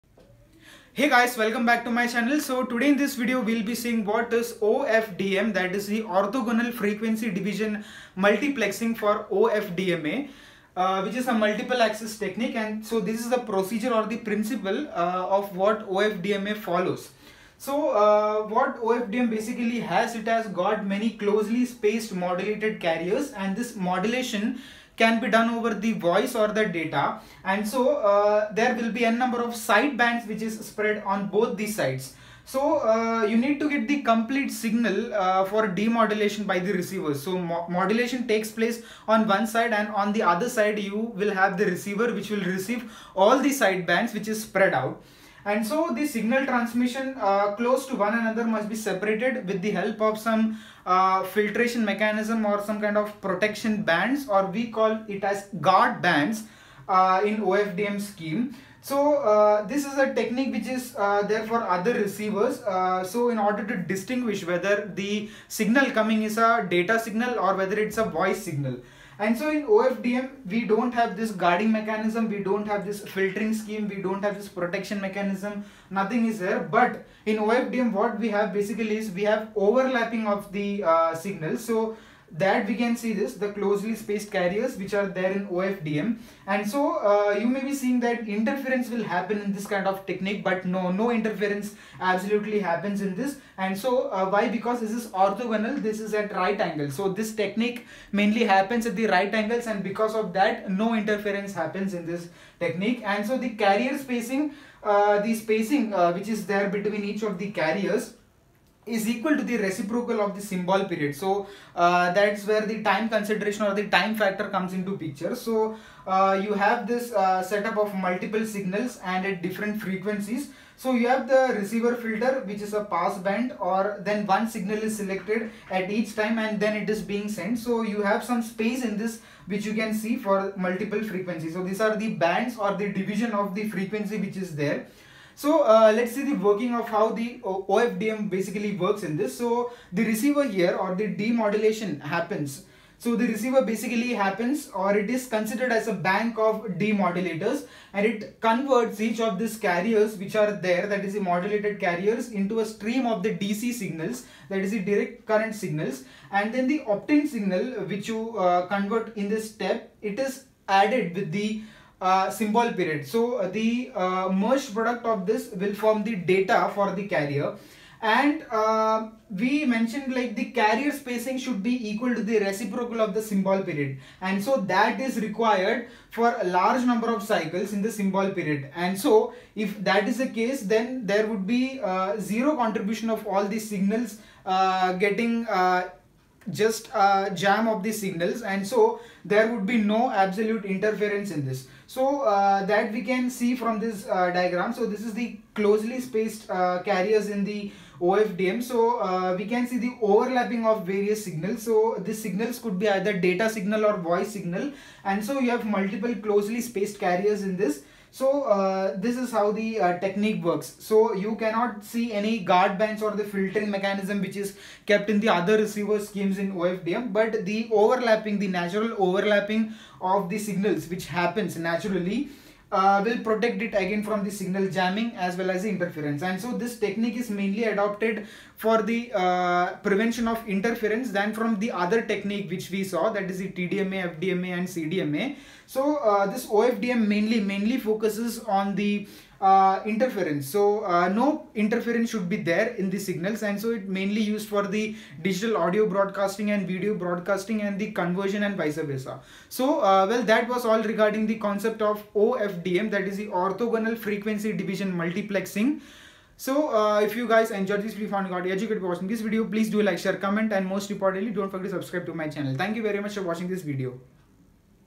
Wireless Technology | Tutorial #10 | Orthogonal Frequency Division Multiplexing
Orthogonal frequency-division multiplexing (OFDM) is a method of digital signal modulation in which a single data stream is split across several separate narrowband channels at different frequencies to reduce interference and crosstalk.